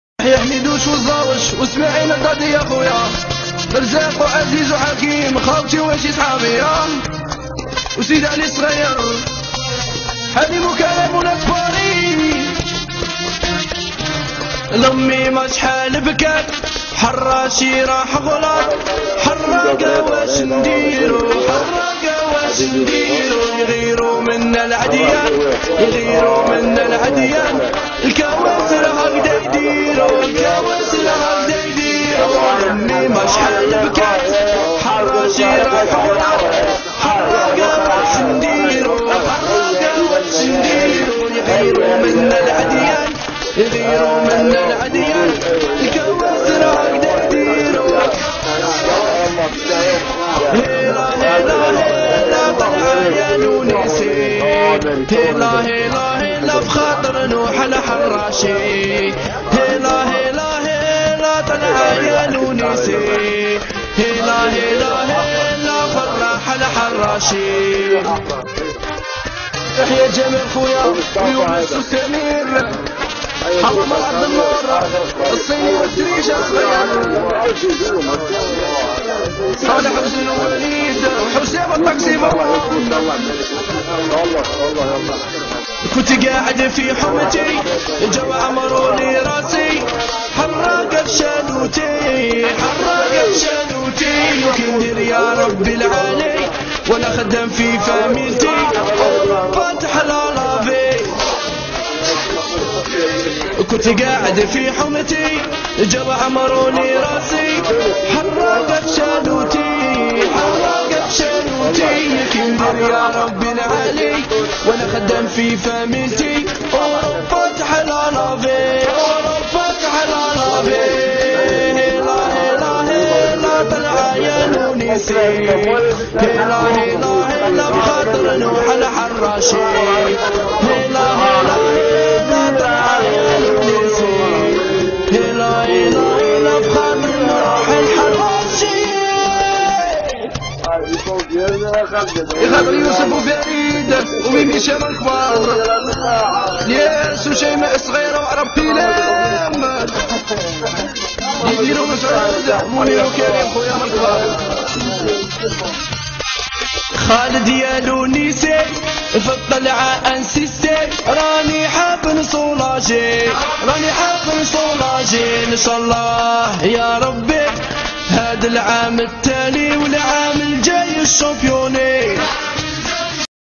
haraga mostaganem arsa lah yarhamhoum
هادوا الحراقة تع العرصة الله يرحمهم و يغفرلهم